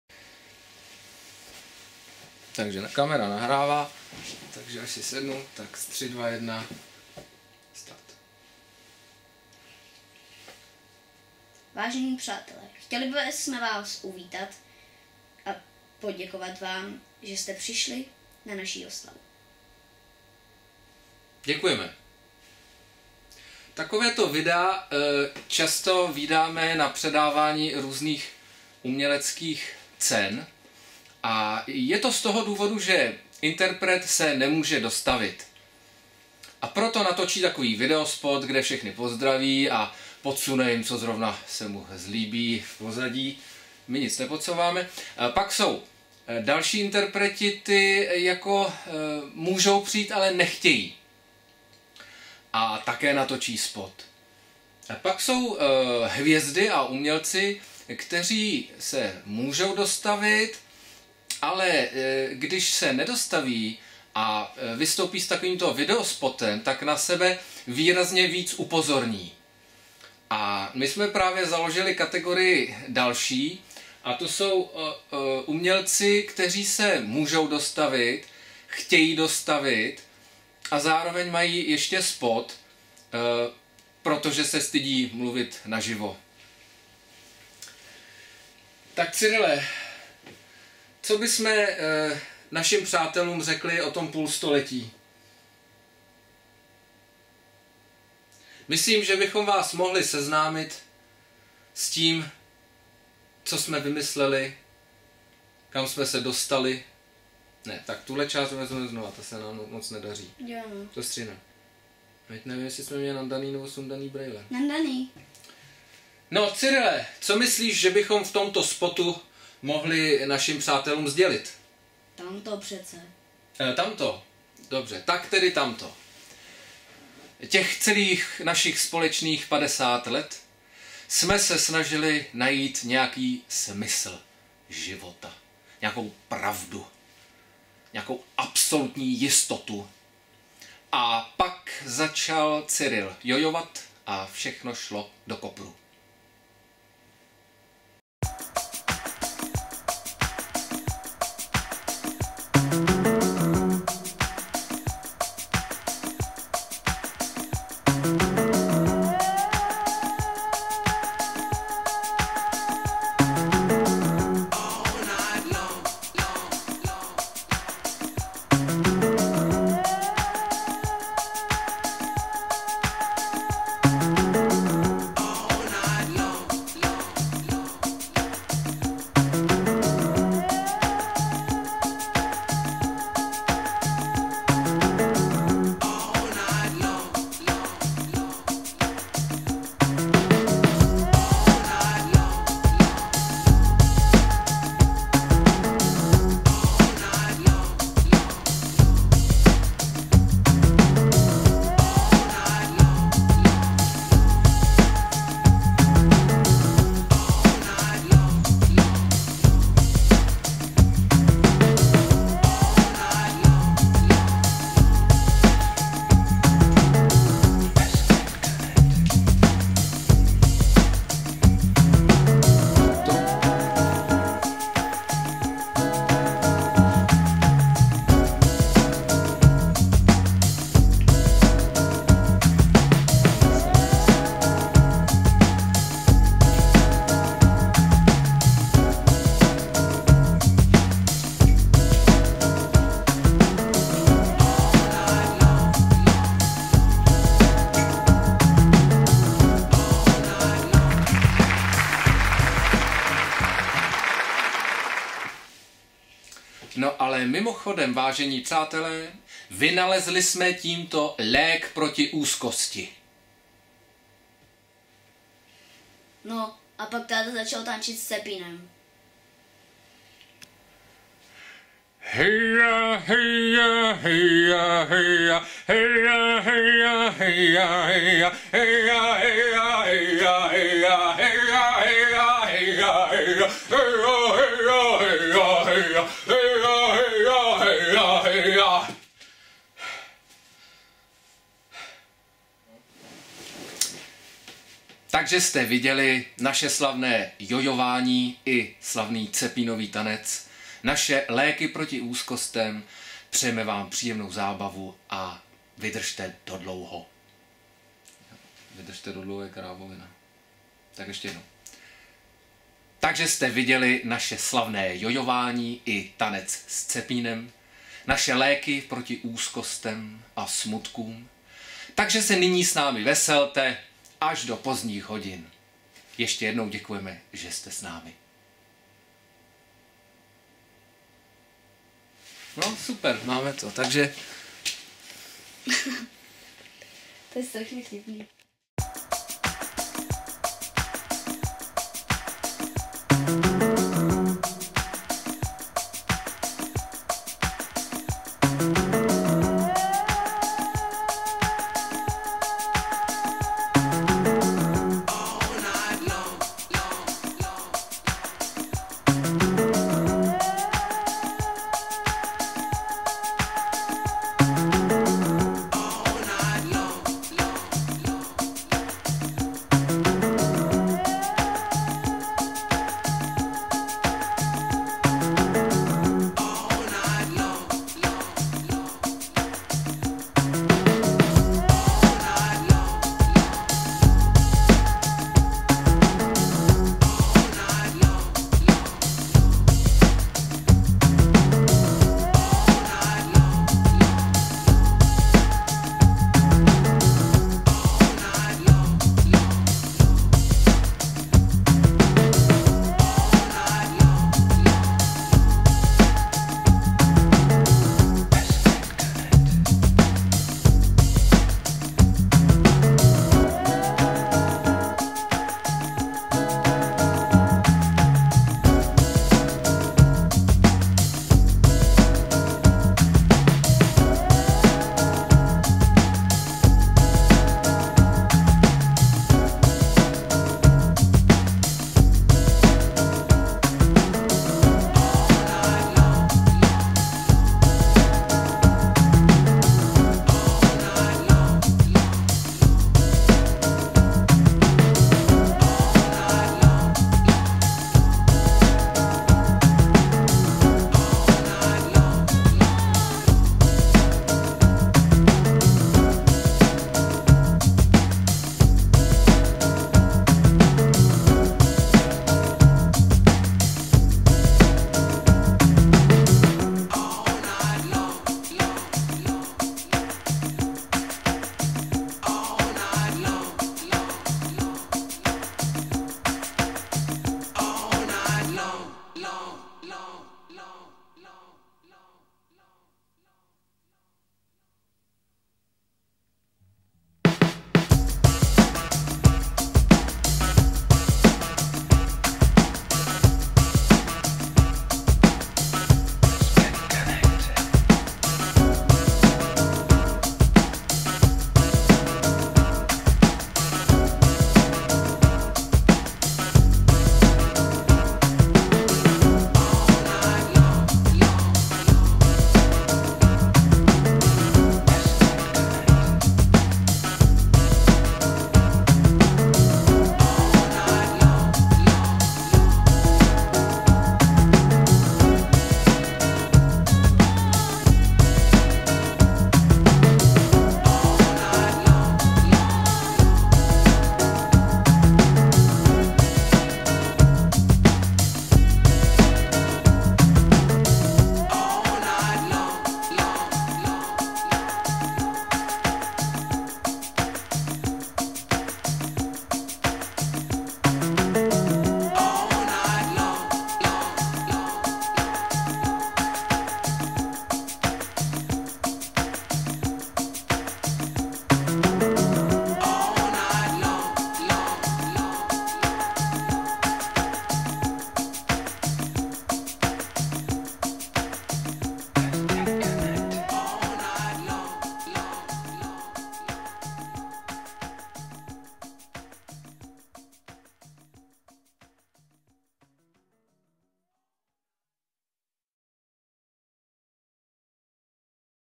zdravice narozky 40+10 1920 x 1080
Štěpán a Cyril Škochovi video spot oslava narozenin 40 a 10